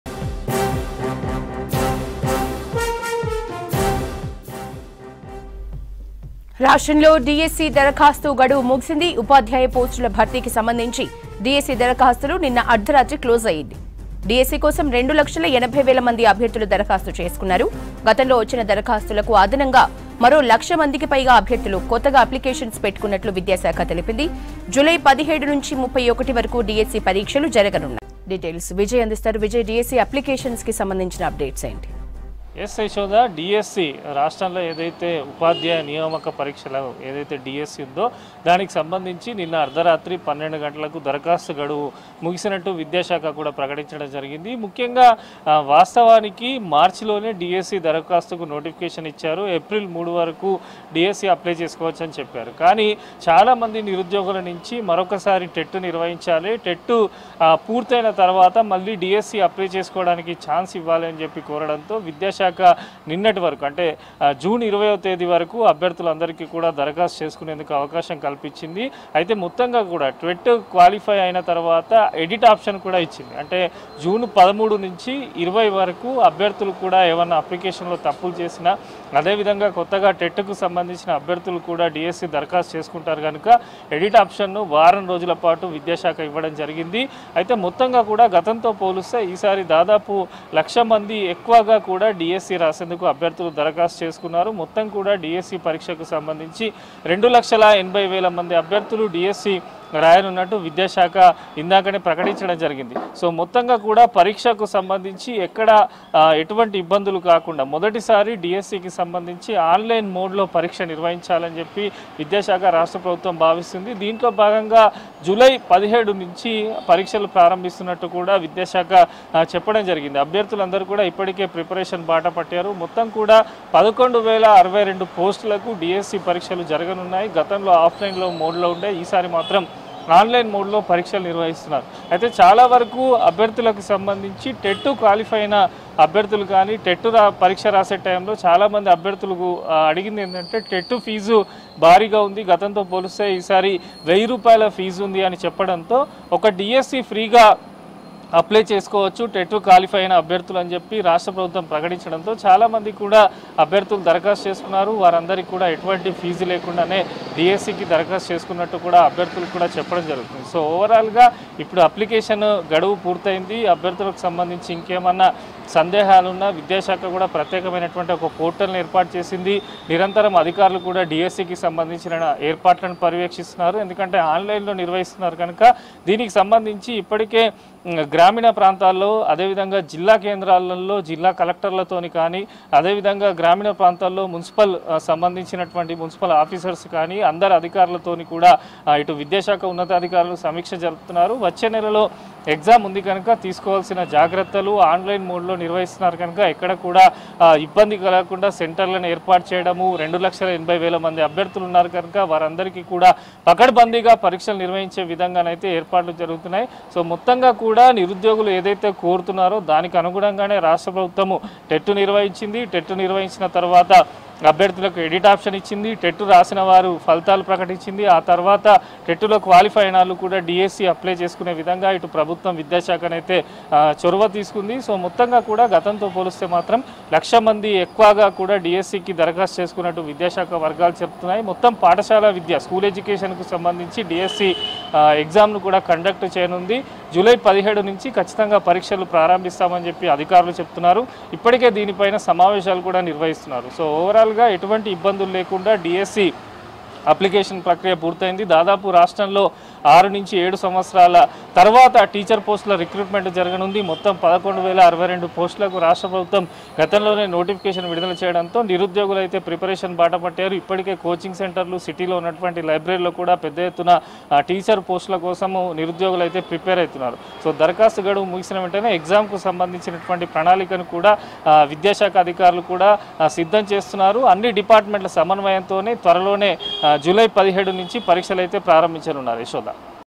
Last Date To Apply DSC Ended In Telangana ,2.80 Lakh Candidates Applied For TG DSC | V6 News
బుర్ఖాలో వచ్చి.. (బంగారం షాపులో కత్తితో బెదిరించడం...బైక్ ఎక్కితే చైర్ తో కొట్టడం)